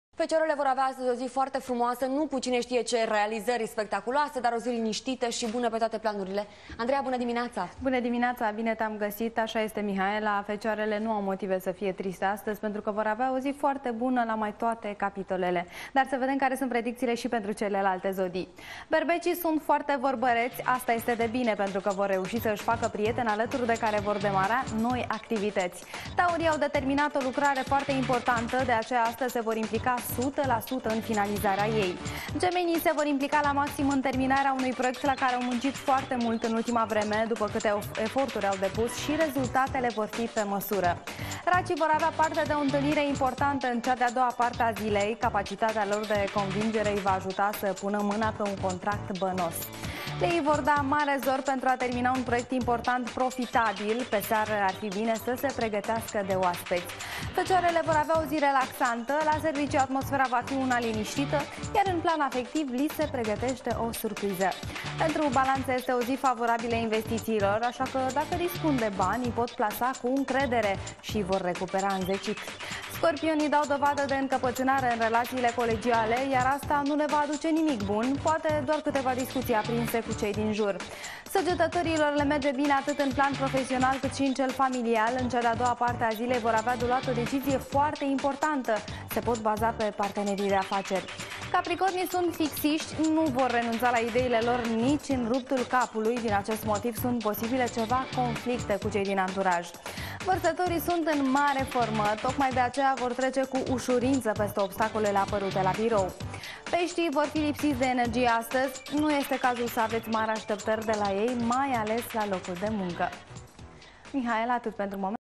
Horoscopul Zilei 16.08.2011 16 AUGUST 2011
Berbecii sunt foarte vorbareti. Asta e de bine pentru ca vor reusi sa isi faca prieteni alaturi de care vor demara noi activitati.
Taurii au de terminat o lucrare impprtanta. De aceea astazi se vor implica suta la suta in finalizarea lui.
Gemenii...